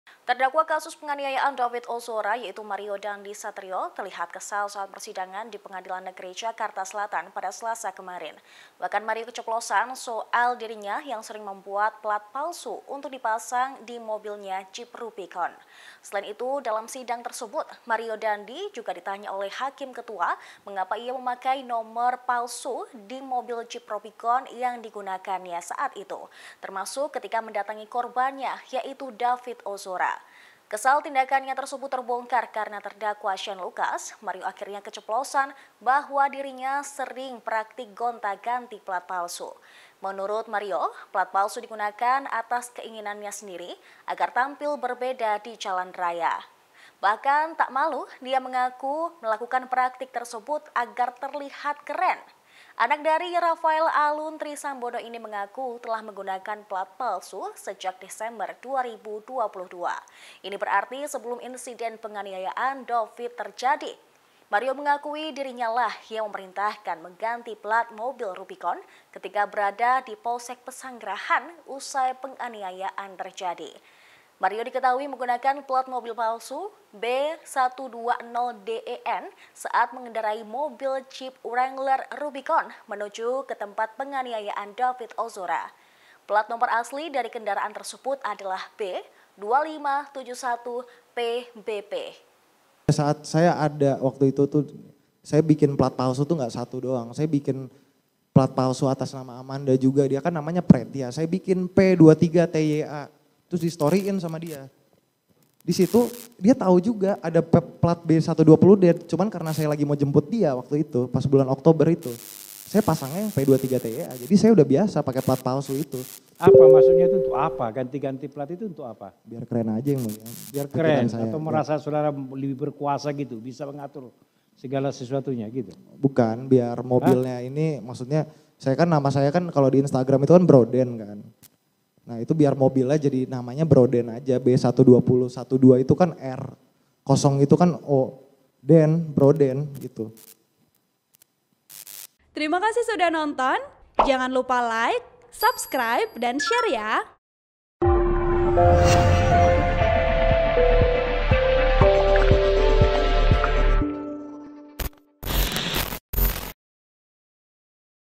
Mario Dandy Kesal hingga KECEPLOSAN NGAKU Sering Bikin Pelat Palsu: Biar Keren Aja yang Mulia
TRIBUN-VIDEO.COM - Terdakwa kasus penganiayaan David Ozora yaitu Mario Dandy Satriyo terlihat kesal saat persidangan di Pengadilan Negeri Jakarta Selatan, Selasa (4/7/2023).

Bahkan Mario keceplosan soal dirinya yang sering membuat pelat palsu unyuk dipasang di jeep Rubiconnya.

Dalam sidang tersebut Mario Dandy ditanya oleh Hakim Ketua mengapa ia memakai pelat nomor palsu di mobil Jeep Rubicon yang digunakannya.

Termasuk saat mendatangi korbannya yaitu David Ozora.

Kesal tindakannya tersebut terbongkar karena terdakwa Shane Lukas, Mario akhirnya keceplosan bahwa dirinya sering praktik gonta-ganti pelat palsu.

Menurut Mario, pelat palsu digunakan atas keiinginanya sendiri agar tampil berbeda di jalan raya.

Bahkan tak malu, dia mengaku melakukan praktik tersebut agar terlihat keren.

Anak dari Rafael Alun Trisambodo ini mengaku telah menggunakan pelat palsu sejak Desember.

Ini berarti sebelum insiden penganiayaan David terjadi.

Mario mengakui dirinya lah yang memerintahkan mengganti pelat mobil Rubicon ketika berada di Polsek Pesanggrahan usai penganiayaan terjadi.

Mario diketahui menggunakan pelat mobil palsu B 120 DEN saat mengendarai mobil Jeep Wrangler Rubicon menuju ke tempat penganiayaan David.

Pelat nomor asli dari kendaraan tersebut adalah B 2571 PBP.